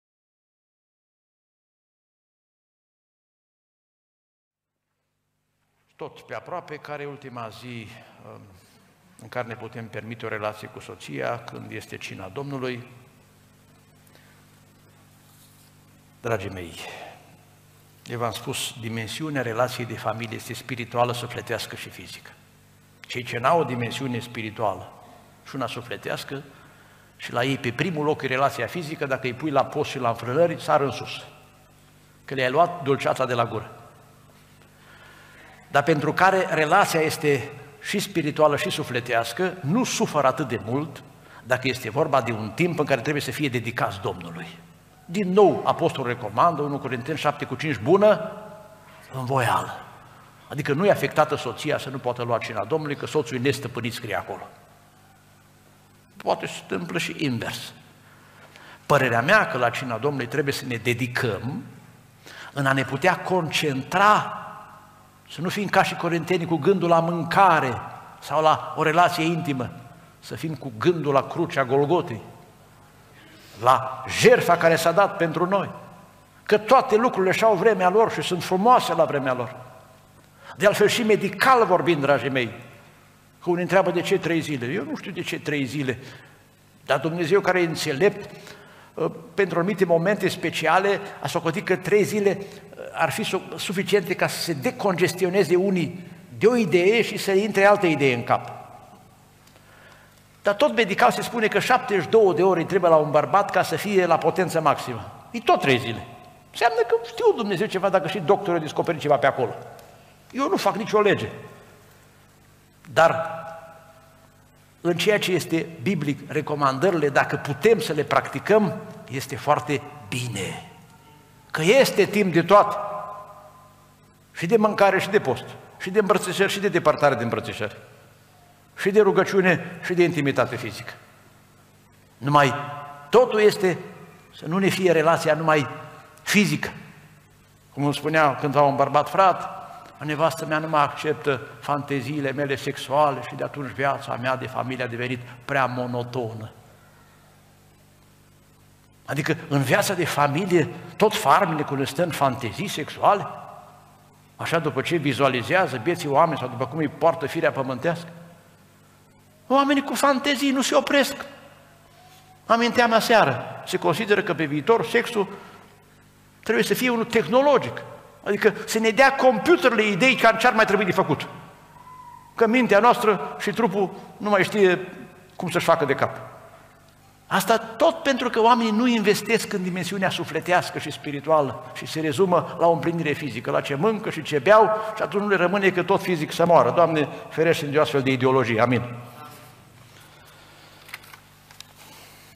Iosif Anca - Abstinența înainte de Cina Domnului
Biserica Penticostală Elim Viena, Austria | 14 ianuarie 2017